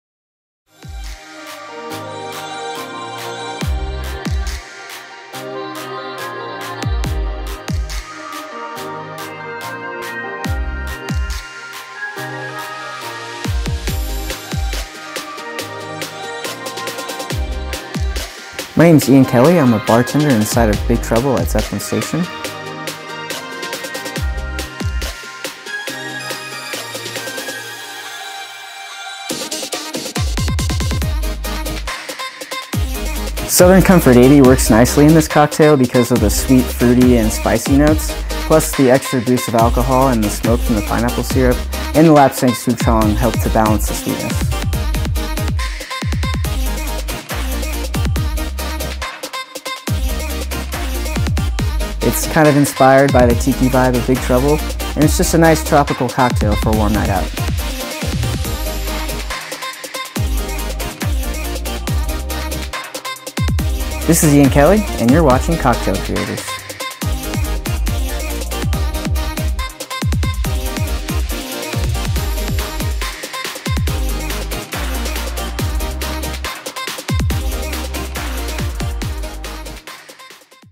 Cocktail Creators: Ian Kelly of Big Trouble
Learn how to make the Denial & Error with Ian Kelly of Big Trouble